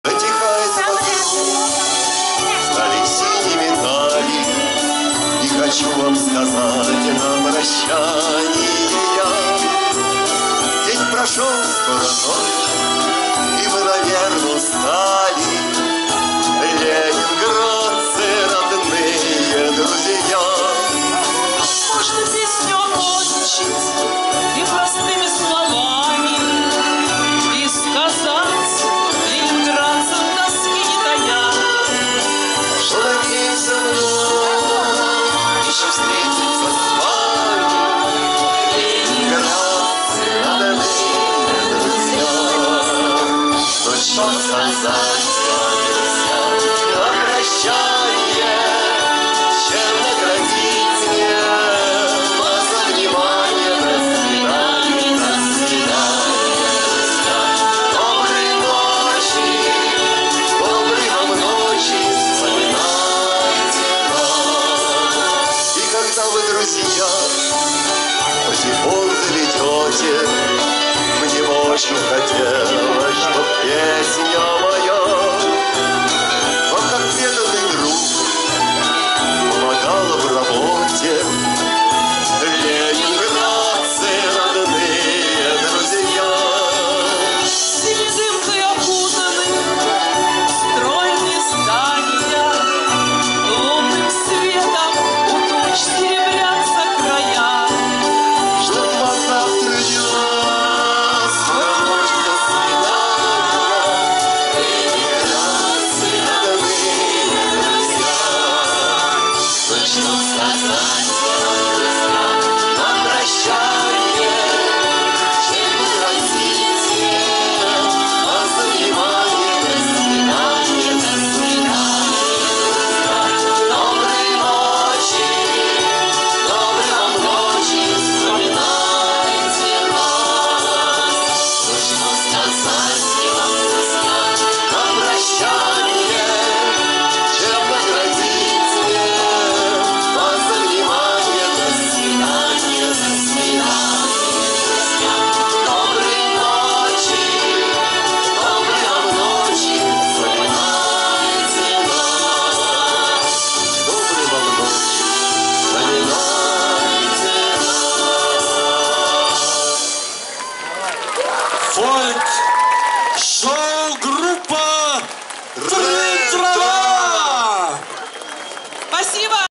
Фольк-шоу-группа "Трынь-трава",Царское село ,9 мая 2018г. ,ЦДМ,
Царское село ,9 мая 2018г. ,выступление муз.коллективов